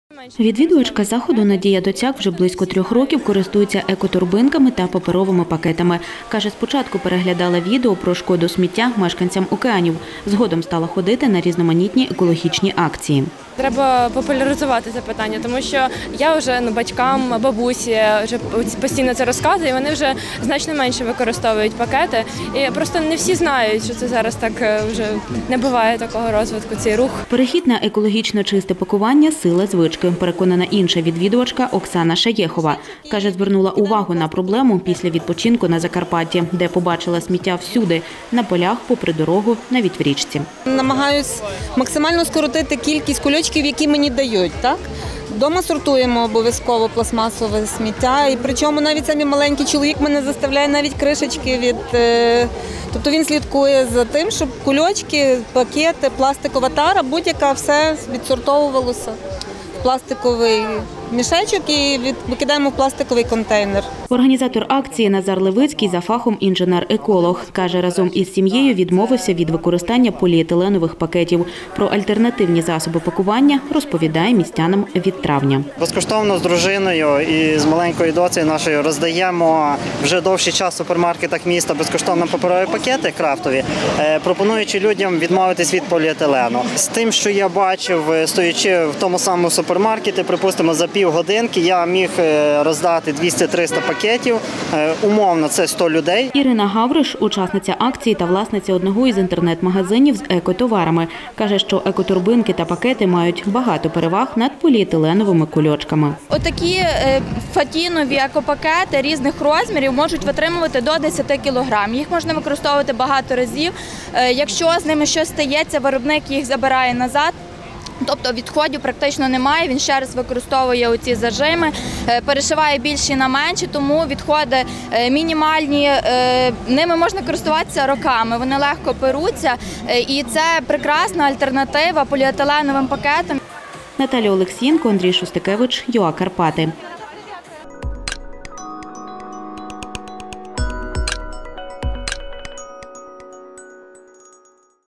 В Івано-Франківську відбулася акція «Ні кульочкам»
Сьогодні на Вічевому майдані в Івано-Франківську провели акцію «Ні кульочкам». Містянам роздавали паперові пакети з вторсировини. Також пропонували купити екоторбинки на благодійному аукціоні. За словами організатора акції Назара Левицького, виручені гроші передадуть у Містечко милосердя святого Миколая.